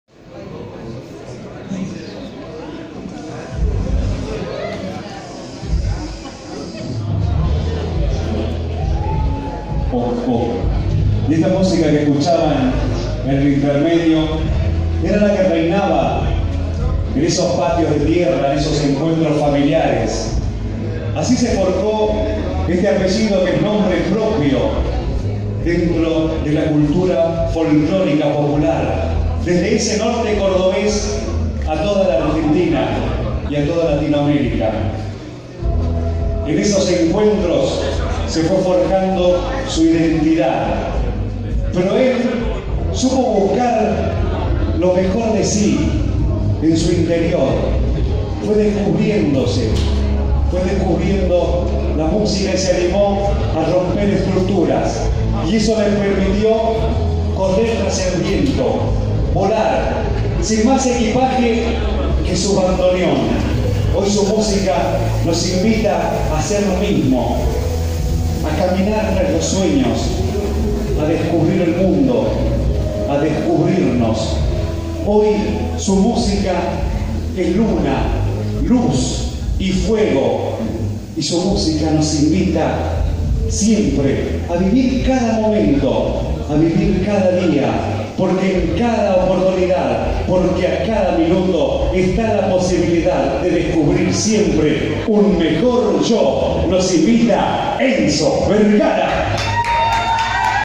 Locutor Julio Cerrizuela en Niceto Club
Presentando la noche de Enzo Vergara en un nuevo evento de Galván Producciones.